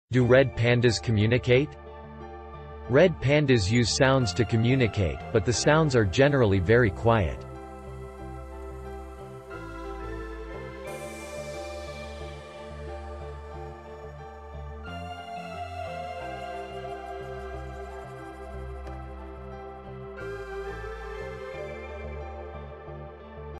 Do red pandas communicate?
Red pandas use sounds to communicate, but the sounds are generally very quiet.

Reference:
International Red Panda Day - September 15 | Smithsonian Science ...